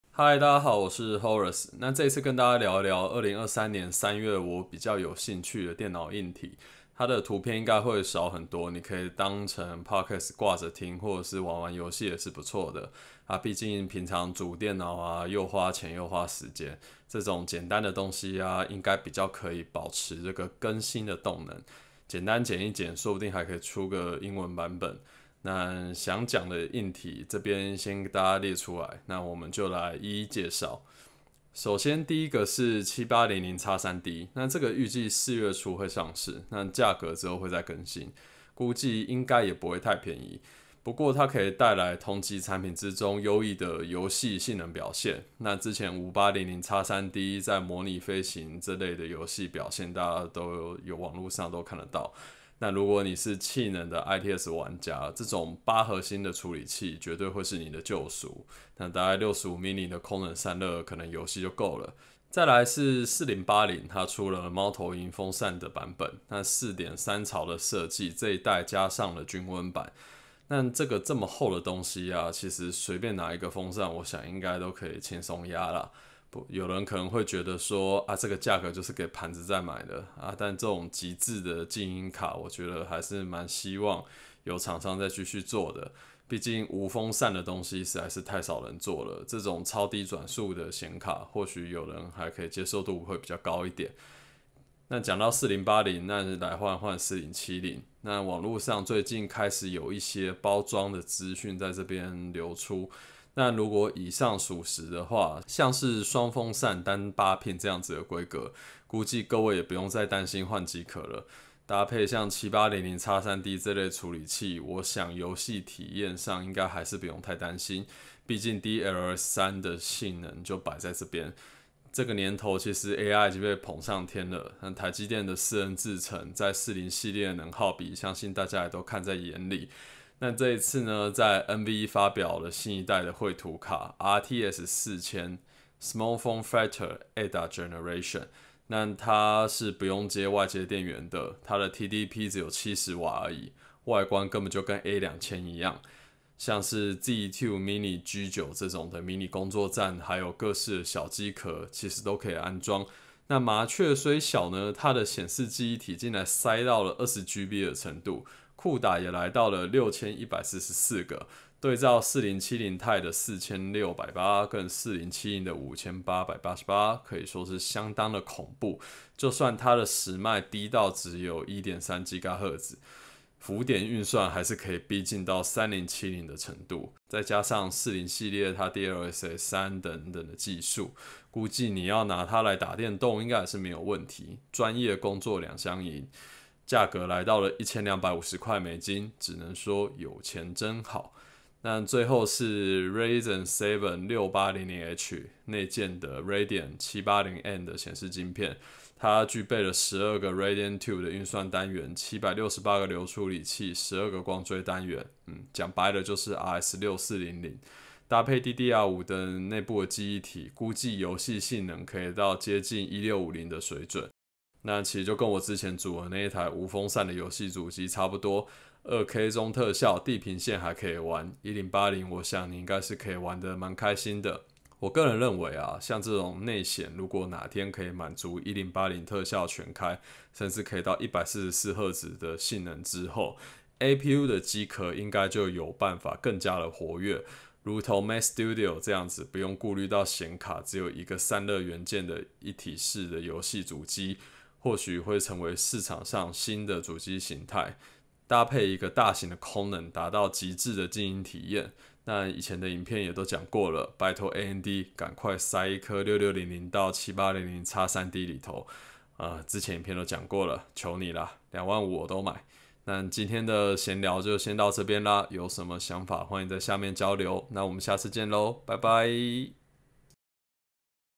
2023三月硬體閒聊，地表最強免插電顯卡降臨
1.7800X3D
2.RTX4080 NOCTUA
3.4070
4.RTX 4000 SFF Ada Generation
5.Radeon 780M